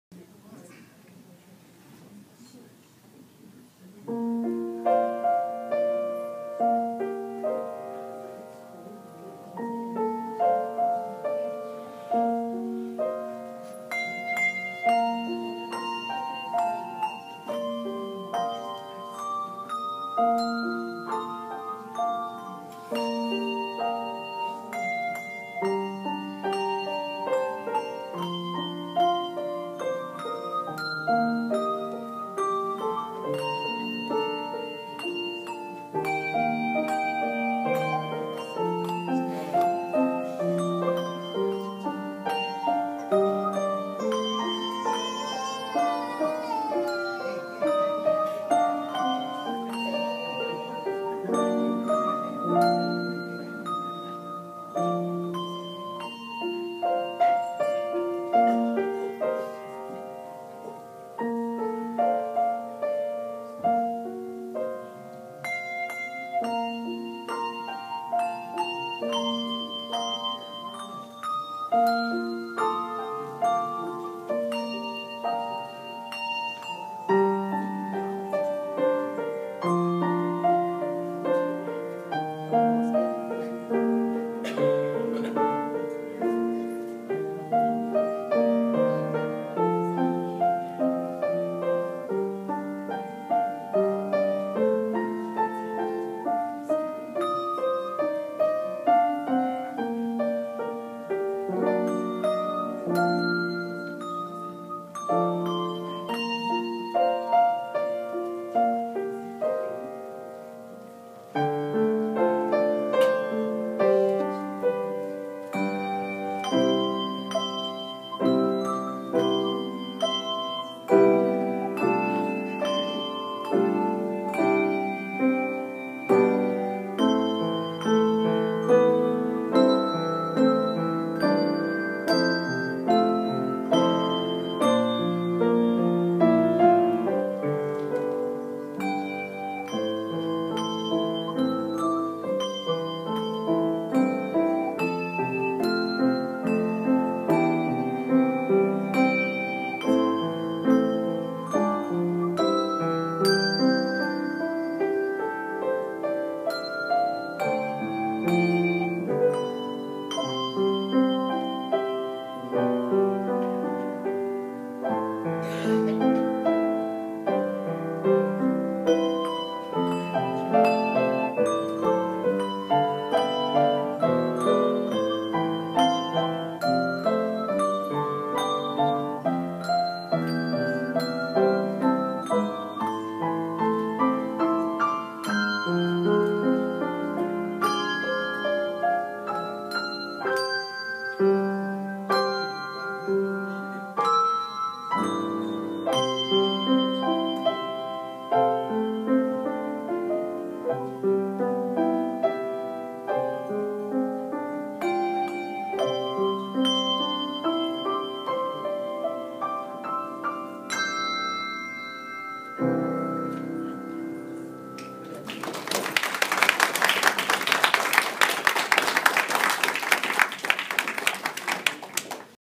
Solo Handbell
Solo performance at Heritage Assisted Living Center, Dec 13 2014